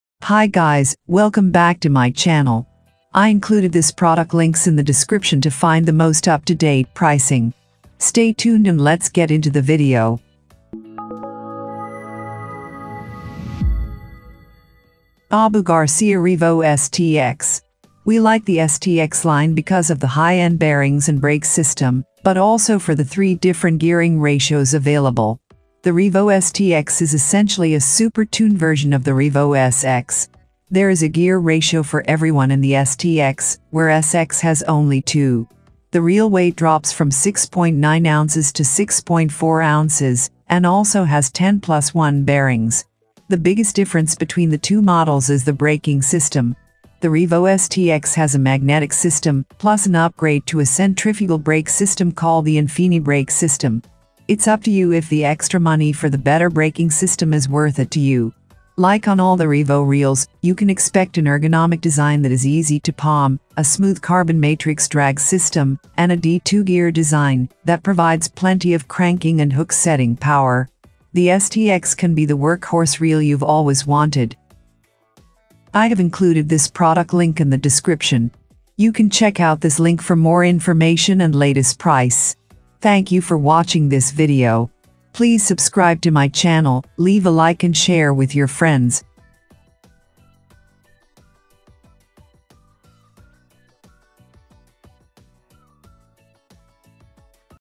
The Best Baitcasting Reel - Abu Garcia Revo STX Review
Abu Garcia Revo STX - Is One Of The Best Baitcasting Reel Of 2021!